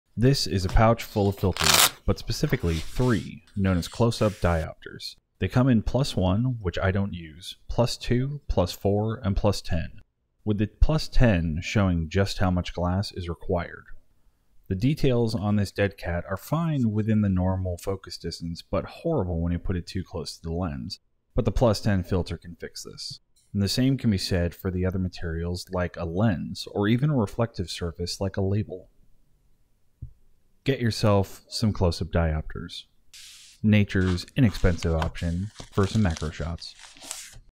Close-Up Filters for Macro Photography & Videography
An inexpensive option for some macro details in your photography and videography... go check these out for camera accessories!